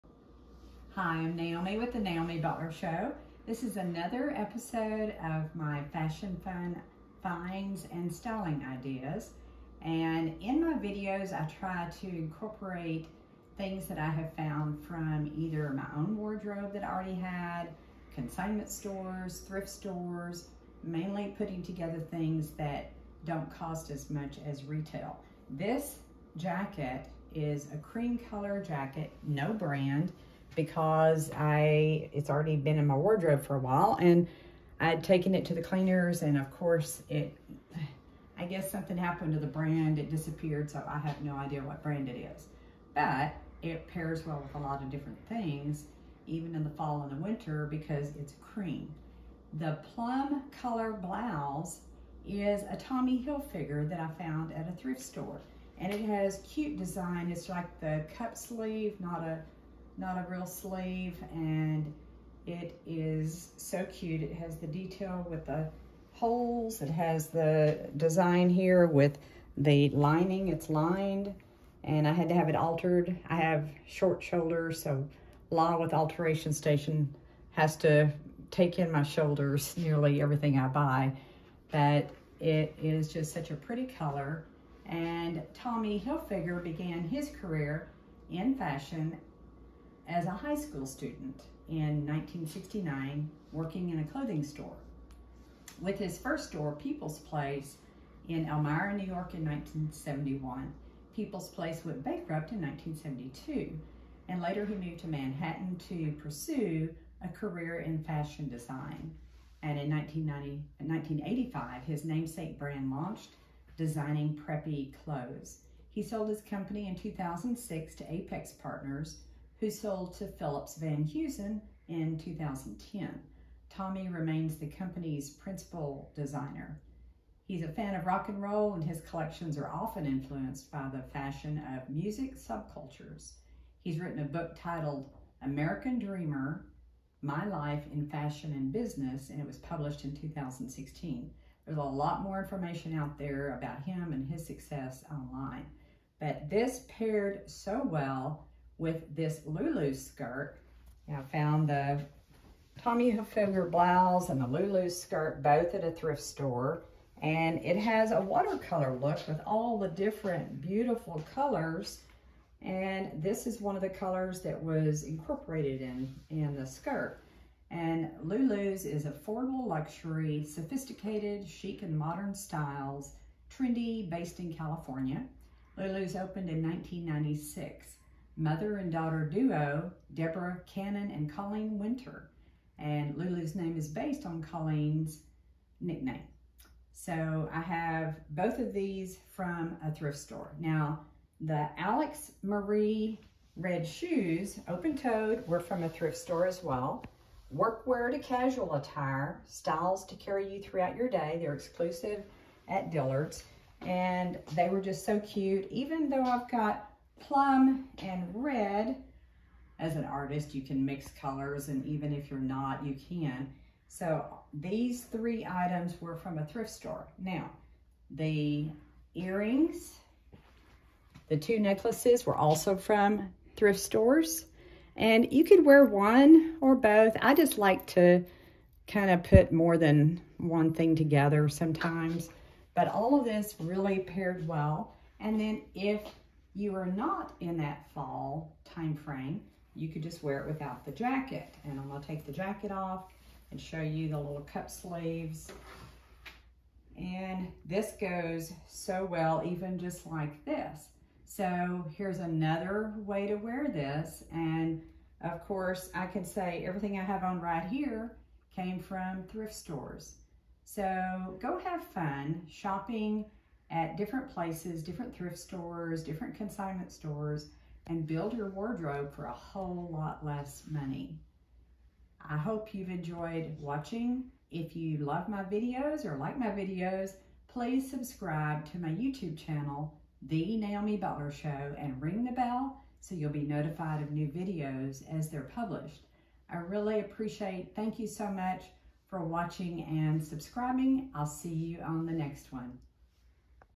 Fashion Fun Finds, Part 20 (Tommy Hilfiger, Lulu's, Alex Marie)
This "Tommy Hilfiger" lined plum blouse was a fantastic find at a thrift store.  I had it altered a little to fit correctly, and it is such a beautiful color.  In this video, you will hear me tell part of his fashion brand story, which is so interesting.
I love the watercolor look of this "Lulu's" skirt, also found at a thrift store.  The skirt has so many yummy colors, plum, purple, red, and all colors in between those.  The cream colored jacket was something that I had already in my wardrobe, and the brand has disappeared from the back, so it is unknown.
"Alex Marie" red open toed shoes (also a thrift store find) complete the look because of the red colors in the skirt.
You can hear the fashion brand stories of these designers in my video.
The jewelry is all from thrift stores as well.
It is so much fun finding things for a lot less than retail, especially nice clothes such as these.
"Tommy Hilfiger" plum blouse from thrift store.
"Lulu's" skirt from thrift store.
"Alex Marie" shoes from thrift store.
Jewelry from thrift store.
Jacket already in my wardrobe.

This outfit really works well with the Valentine theme for this month of February.

You will find videos related to other fashion, as well as art, cancer, cooking, business, doll clothes, and more.